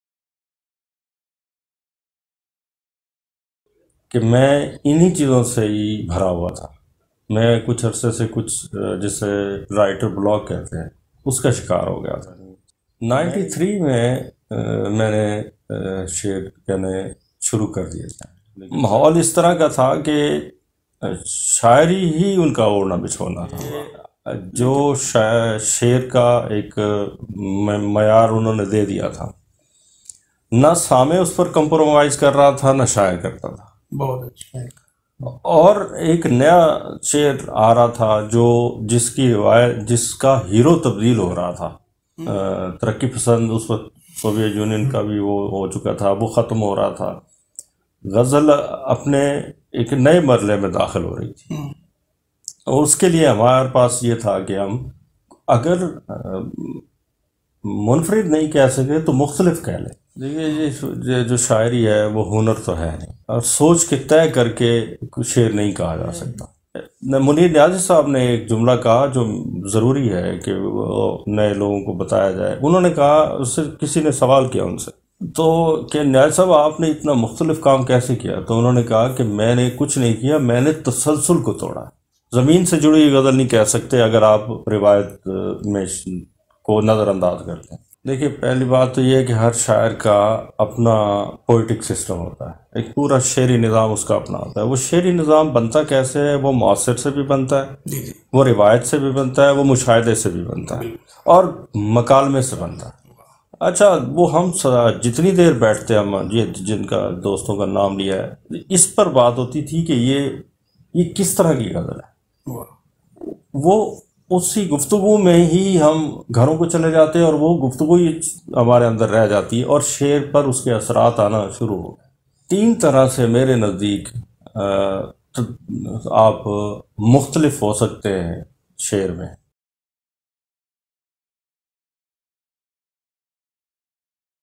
TEASER..Faisal Hashmi (Interview)
Faisal Hashmi is prominent poet of contemporary Urdu Literature. He influenced many young poets. His book was awaited since last one and half decades